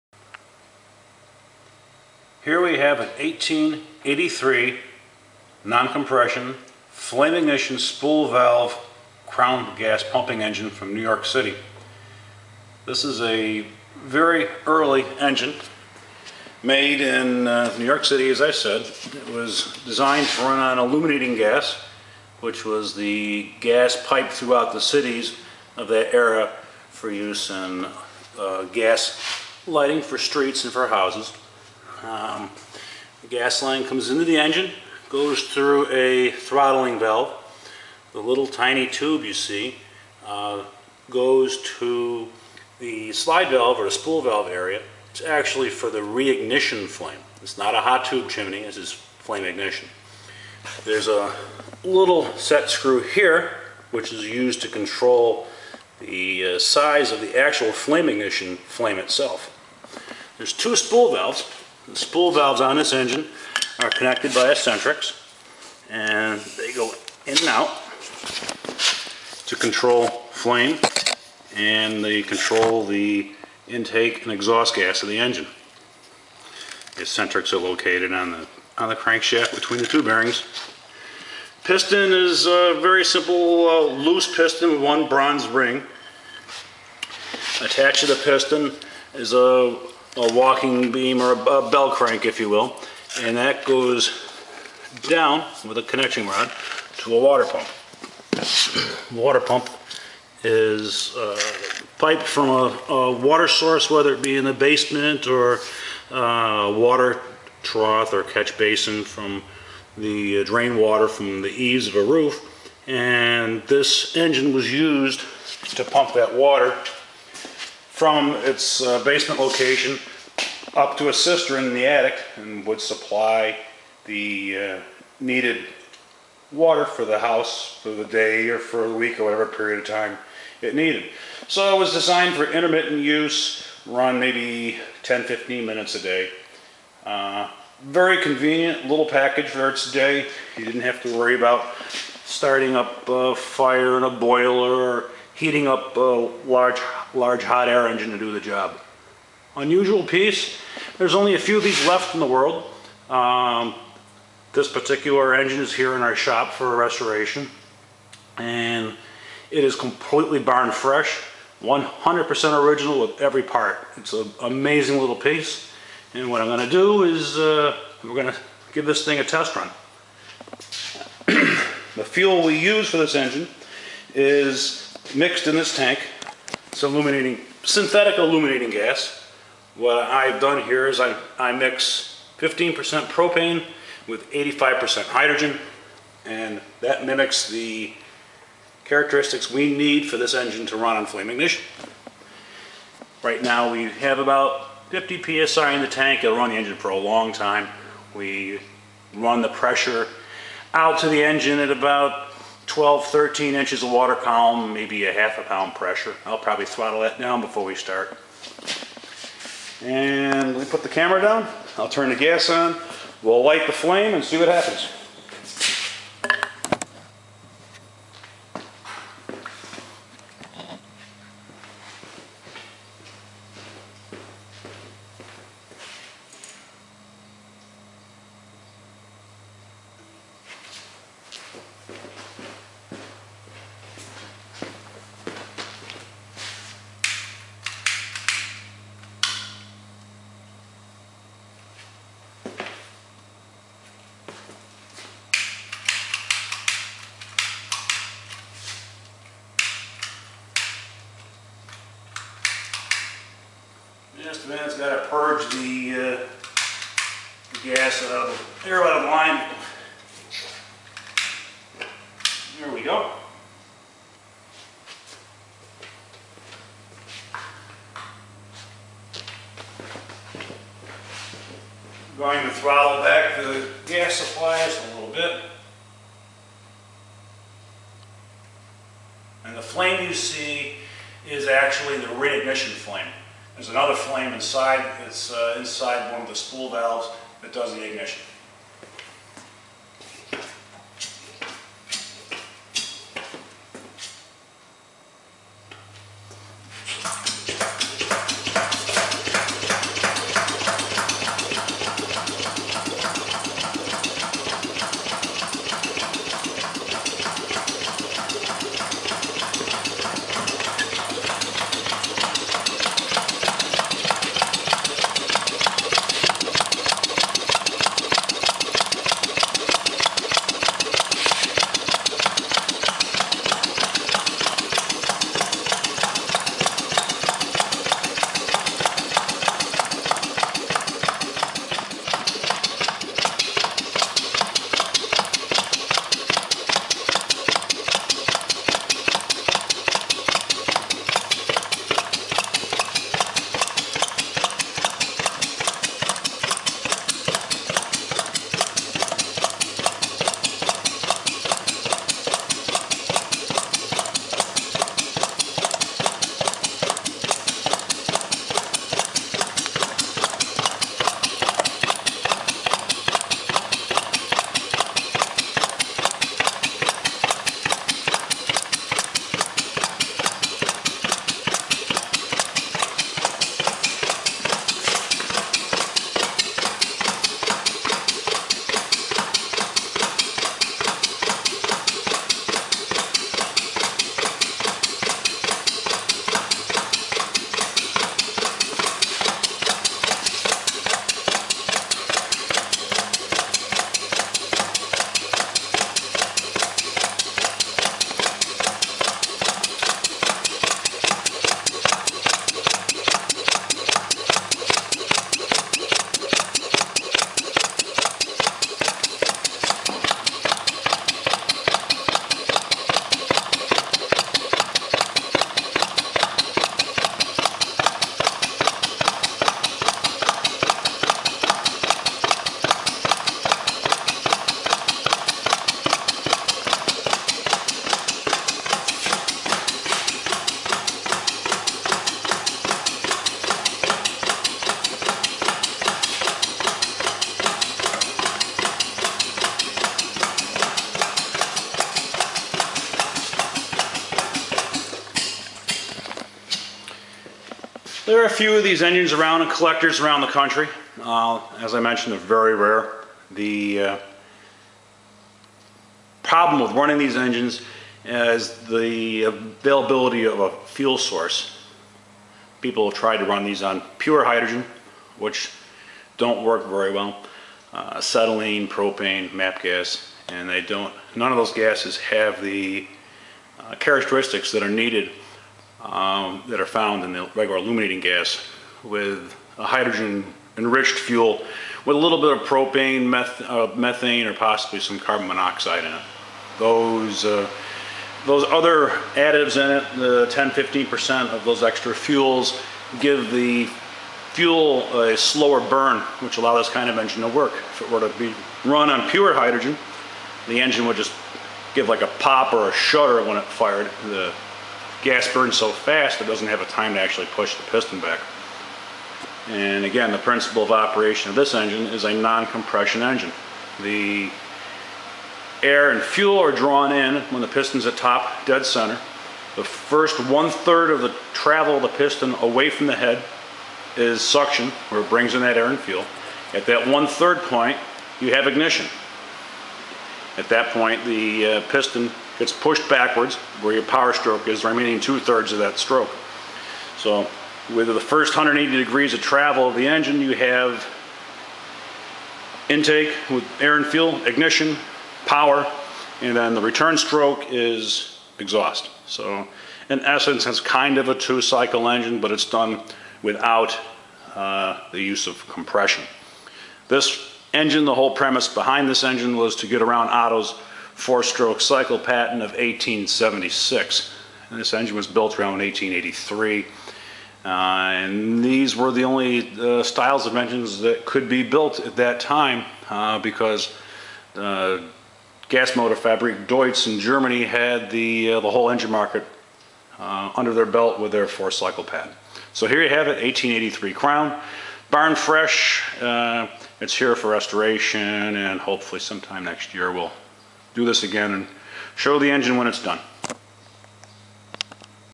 1883 Crown Pumping Engine - Flame ignition Non-Compression
1883 Crown Pumping Engine, Barn Fresh - Amazingly Complete and original.  This engine was designed to run on "illuminating gas" and operates on a non-compression cycle.  Built in New York City its purpose was to run a few minutes a day to pump water from a catch basin ( filled by roof collected drain water)  in the basement to a cisterns in the attic.   These are very rare and only a handful survive today.  This particular engine is running on a synthetic illuminating gas comprised of 15% propane 85% hydrogen. The water it pumps travels through the engine cylinder water jacket prior to exiting the side of the pressure dome (on top).
The Crown Pumping engine was built by The National Meter Co. of NYC and designed by Lewis Hallock Nash